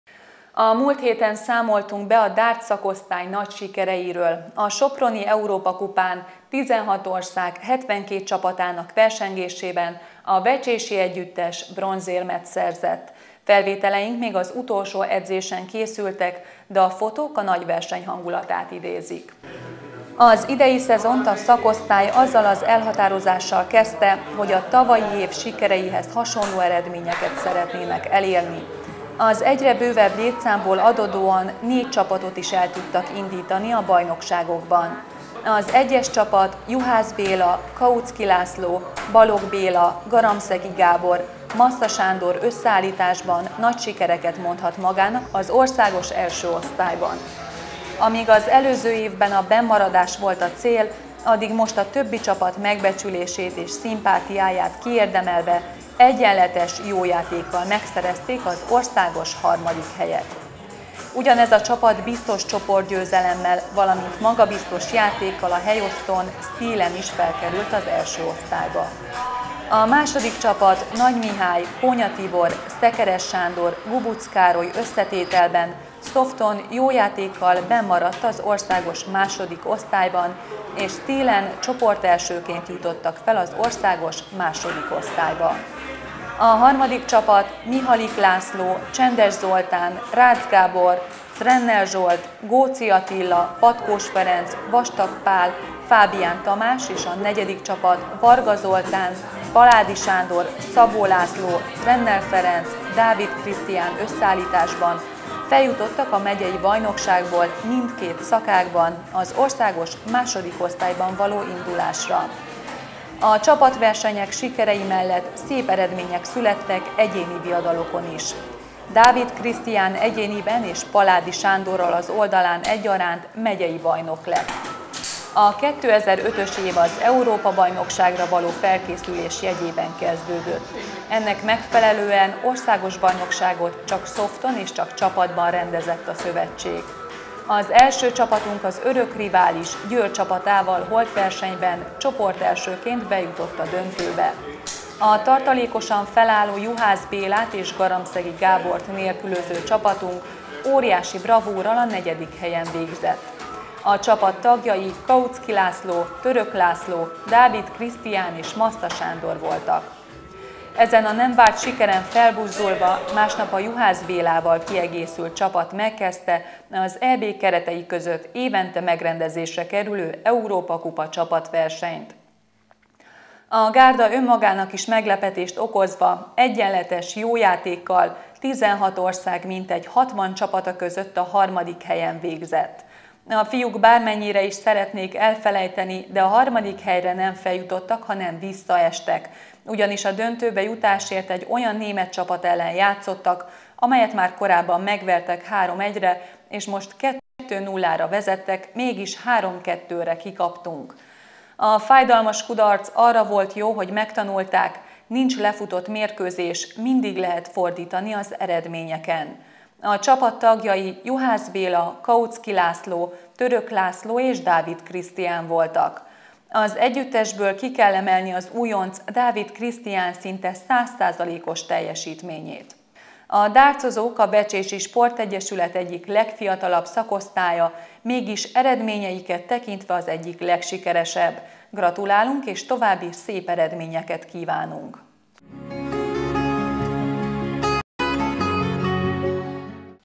5 109 Vecsési Körkép Retro 2005 augusztus Darts
A múlt héten számoltunk be a Darts szakosztály nagy sikeréről. A soproni Európa kupán 16 ország 72 csapatának versengésében a vecsési együttes bronzérmet szerzett!
Felvételeink még az utolsó edzésen készültek, de a fotók a nagy verseny hangulatát idézik.